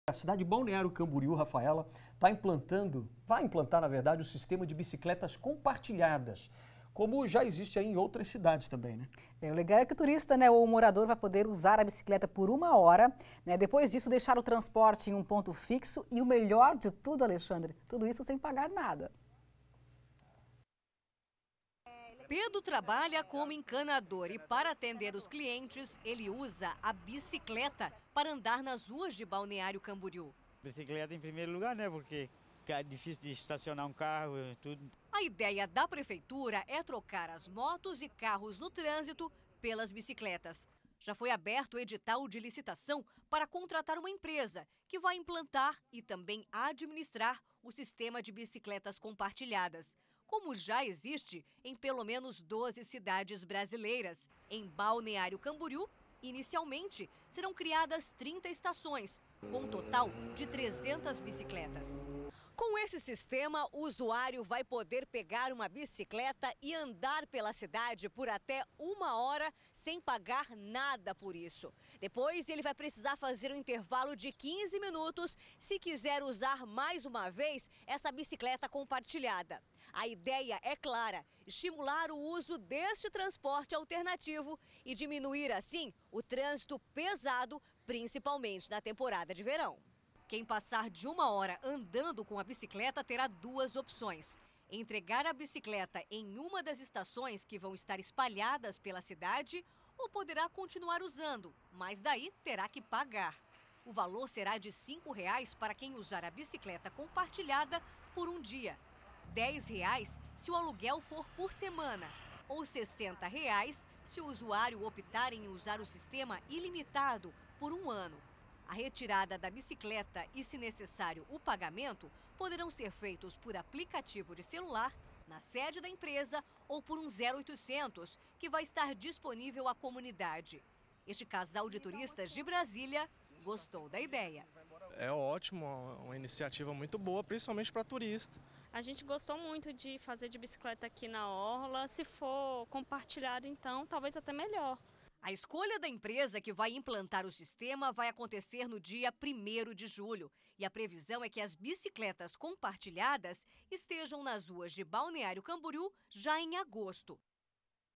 Balneário Camboriú terá sistema de bicicletas compartilhadas
Nesta terça-feira, a prefeitura de Balneário Camboriú lançou um edital para implantar um sistema de bicicletas públicas compartilhadas. A continuidade do projeto era aguardada desde dezembro do ano passado quando ele foi aprovado por lei. O sistema ira permitir que moradores e turistas usem as bikes gratuitamente por até uma hora. A empresa que vencer a licitação deverá instalar 30 estações com 10 bicicletas cada em pontos estratégicos da cidade.